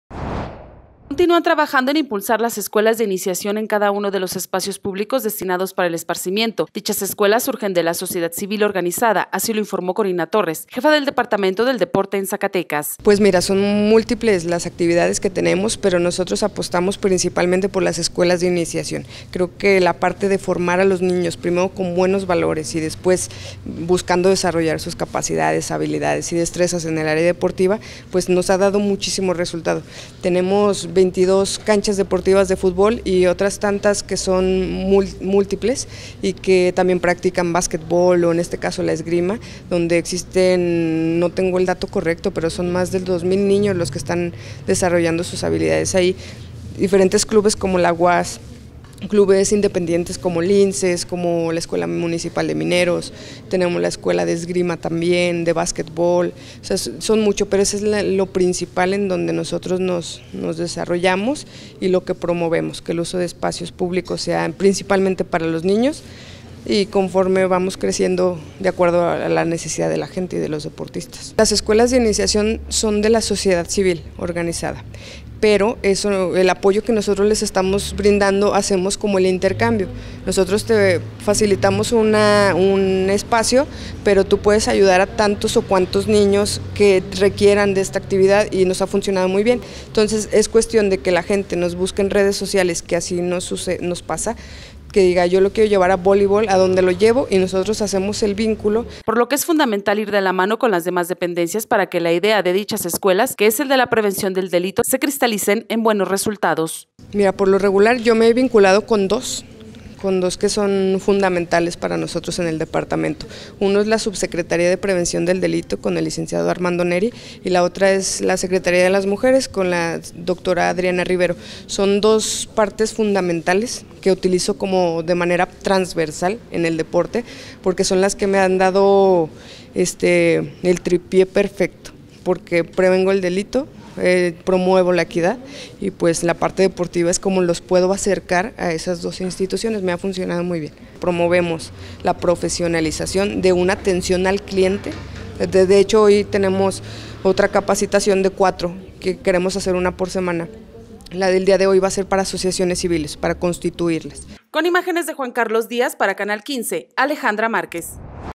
Escuelas de iniciación una de las principales prioridades en la capital zacatecana
Continúan trabajando en impulsar las escuelas de iniciación en cada uno de los espacios públicos destinados para el esparcimiento, dichas escuelas surgen de la sociedad civil organizada, así lo informó Corina Torres Jefa del Departamento del Deporte en Zacatecas.